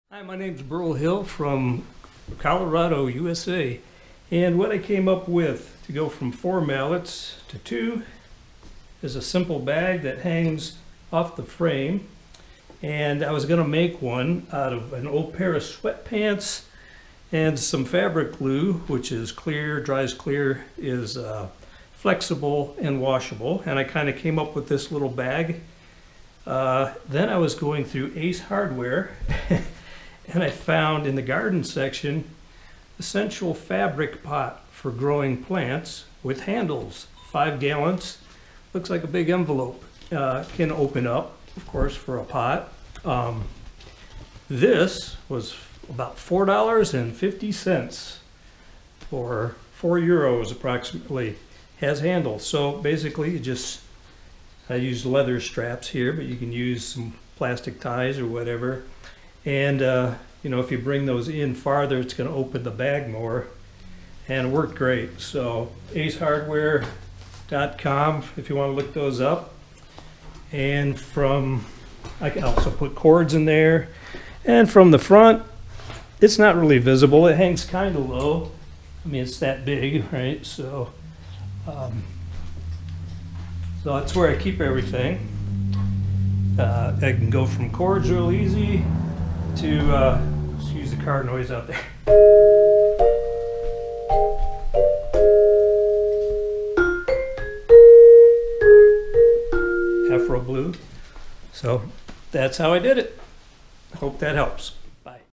Mallet Bag for vibraphone or marimba players
A way to go from four to two mallets quickly and quietly